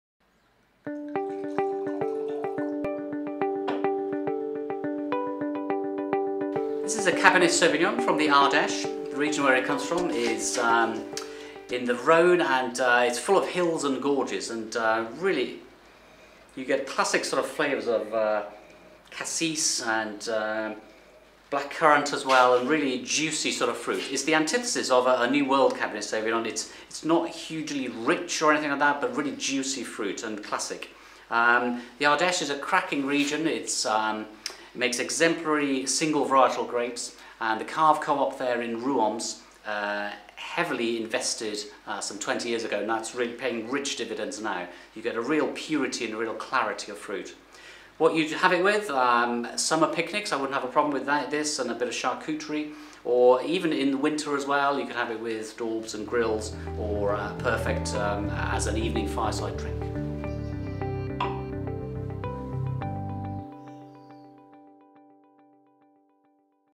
Coteaux de l'Ardèche: Cabernet Sauvignon - Yapp Brothers One Minute Review
Meirion Williams reviews the Coteaux de l'Ardèche: Cabernet Sauvignon - a classic, juicy, fruity wine ideal with summer picnics or winter grills.